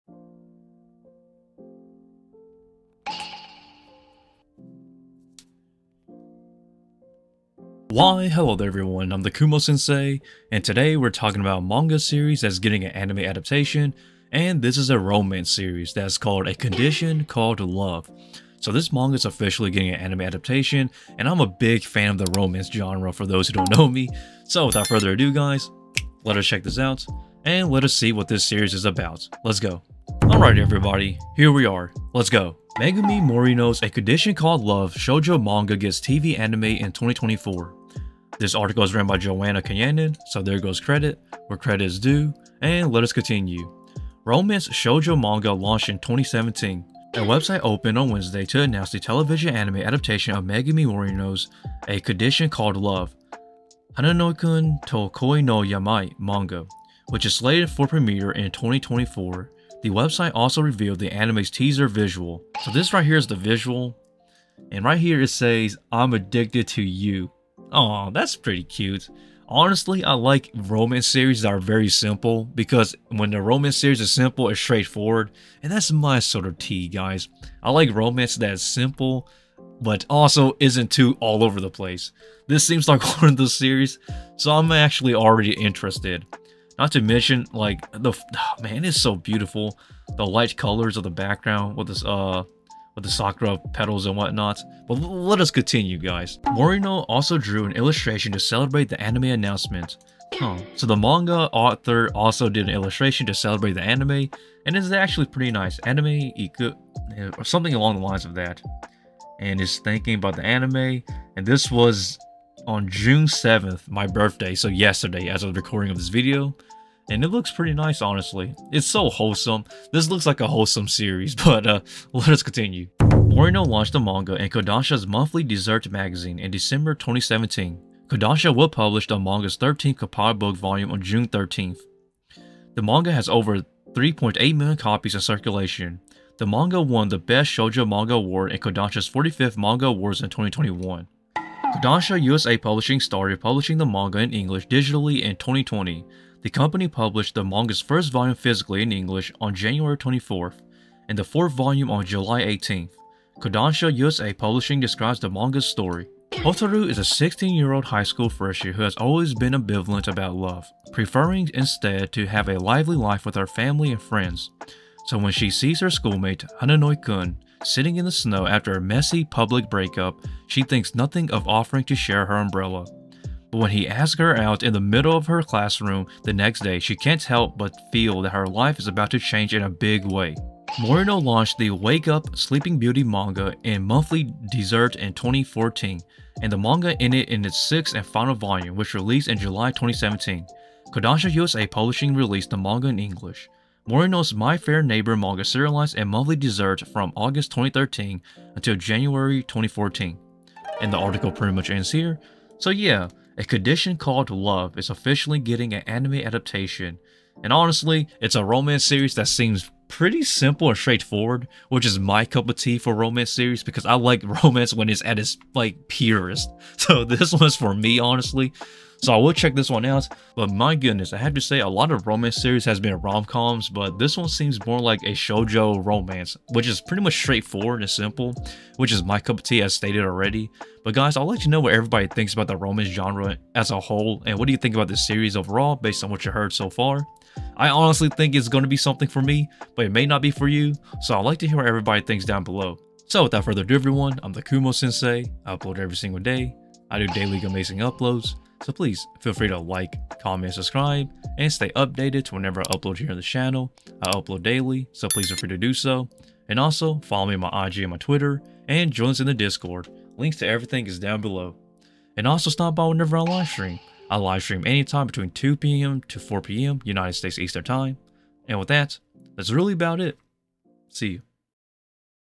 Manga Series - A Condition Called Love Gets Anime CONFIRMED!
Let's talk about Hananoi-kun to Koi no Yamai also known as A Condition Called Love is getting an anime adaptation. This is a shojo romance series that has recently announced that an anime is in the works. So let's talk about this and let's break it down!